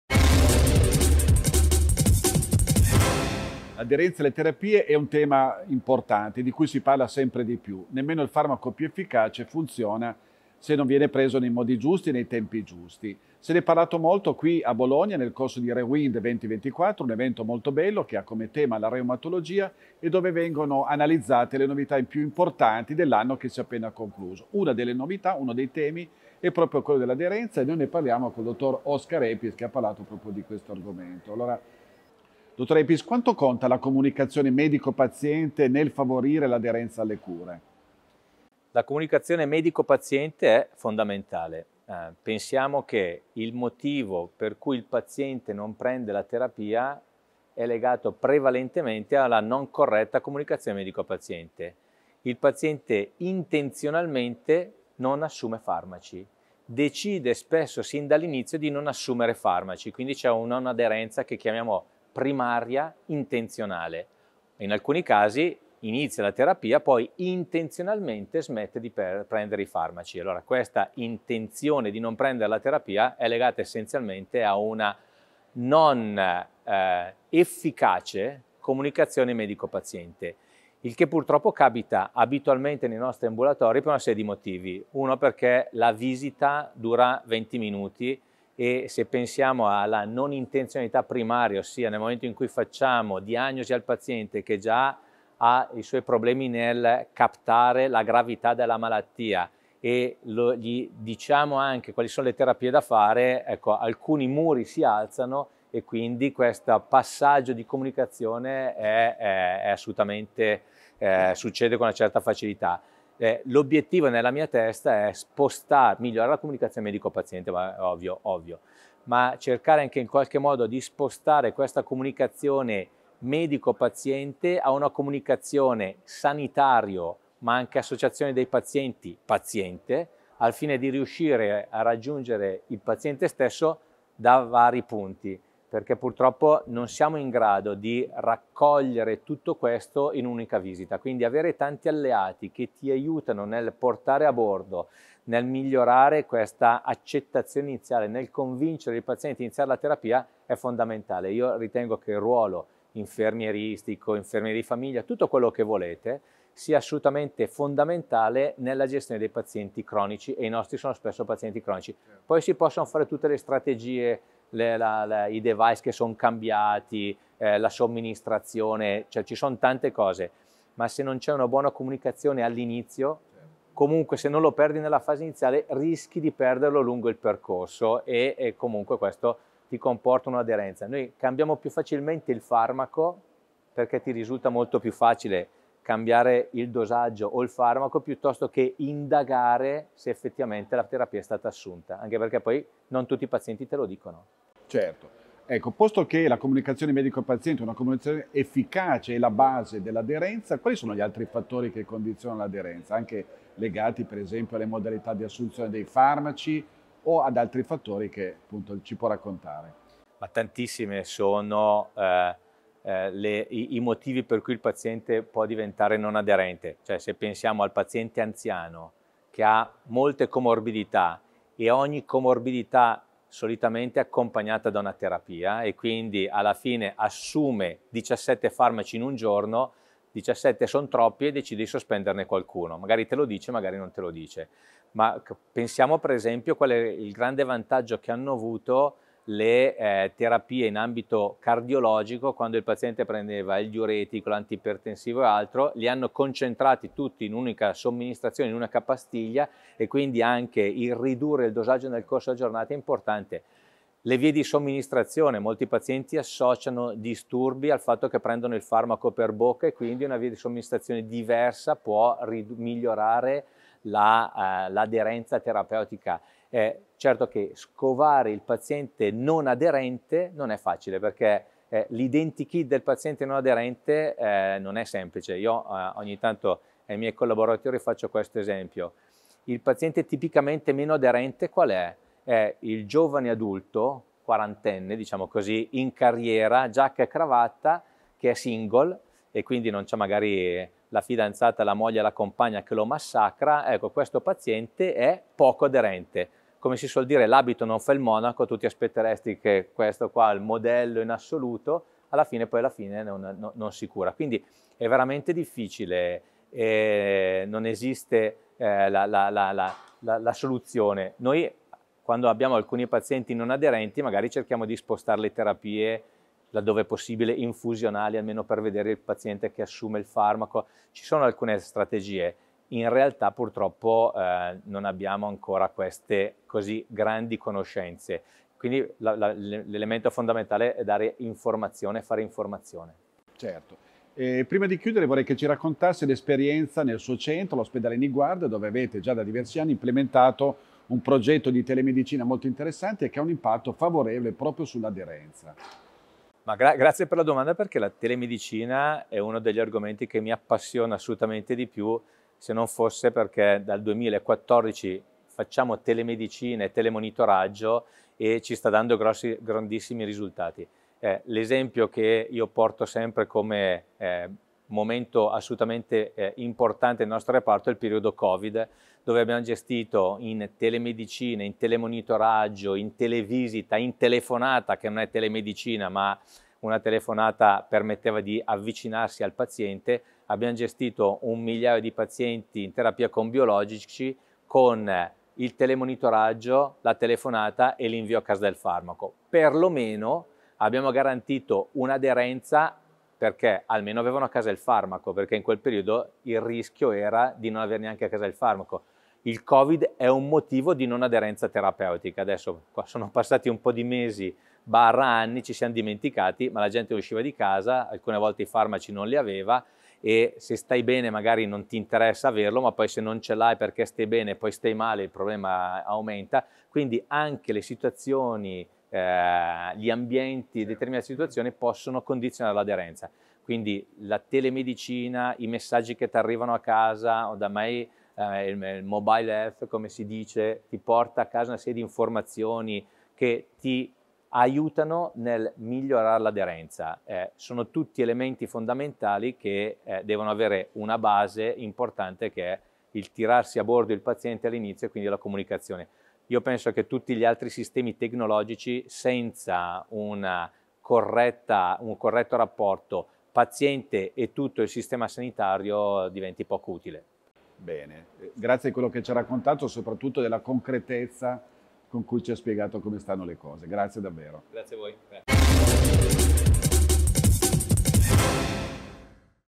Aderenza alle terapie: ruolo della comunicazione medico e paziente e della telemedicina
L'aderenza alle terapie è un tema importante di cui si parla sempre di più. Nemmeno il farmaco più efficace funziona se non viene preso nei modi giusti e nei tempi giusti. Se n'è parlato molto qui a Bologna nel corso di RHEWIND 2024, un evento che ha come tema la reumatologia e dove vengono analizzate le novità più importanti dell'anno che si è appena concluso. Uno dei temi è proprio quello dell'aderenza e noi ne parliamo col dottor Oscar Epis, Direttore Dipartimento Medico Polispecialistico ASST Grande Ospedale Metropolitano Niguarda.